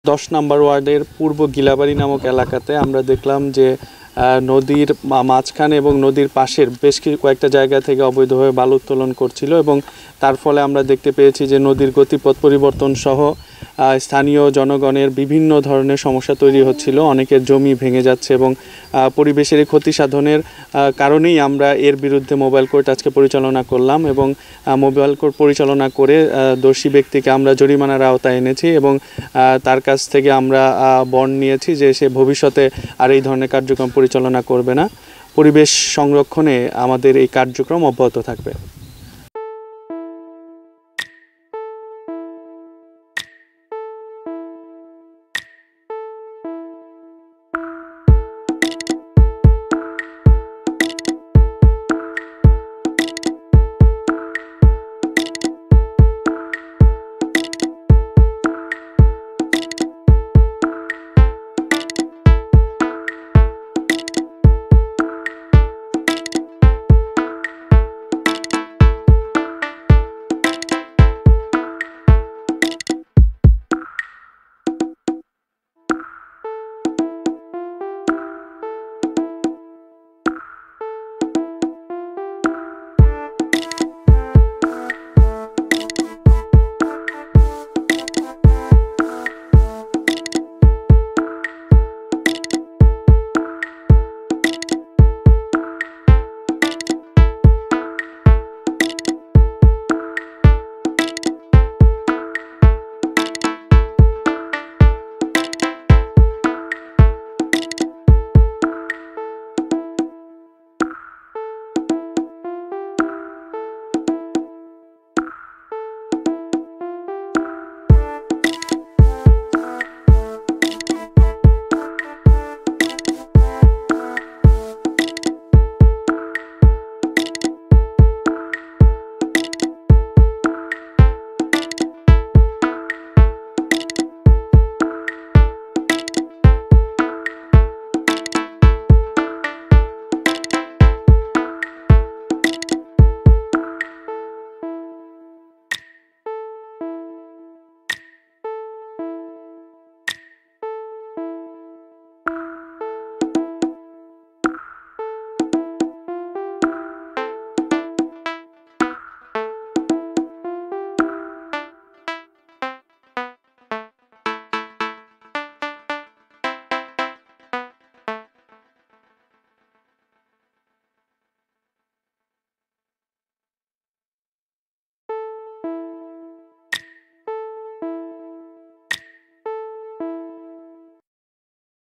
রংপুরে বালু উত্তোলন বন্ধে ম্যাজিস্ট্রেট মৃধার অভিযান
রংপুর নগরীতে কোনো ভাবেই থামছে না মাটি ও বালু উত্তোলন। এরেই প্রেক্ষিতে বালু উত্তোলন বন্ধে বৃহস্পতিবার বিকেলে রংপুর সিটি কর্পরেশন নগরীর ১০ নং গিলাবাড়ী এলাকায় ভ্রাম্যমাণ আদালত পরিচালনা করে। রংপুর মেট্রোপলিটন পুলিশের একটি চৌকস টিমের সহযোগিতায় এ অভিযানে নেতৃত্ব দেন রসিক নির্বাহী ম্যাজিস্ট্রেট মাহমুদ হাসান মৃধা। এসময় একটি বালবাহী ট্রাক্টর জব্ধ ও এক ব্যবসায়ীর ৫ হাজার টাকা জরিমানা করে ভ্রাম্যমাণ আদালতের নির্বাহী ম্যাজিস্ট্রেট। পরিবেশের ভারসাম্য ও ফসলি জমি রক্ষা এবং নদী বাঁচাতে এঅভিযান অব্যাহত থাকবে বলেও জানান রসিকের এই নির্বাহী কর্মকর্তা
C O N N E C T W I T H N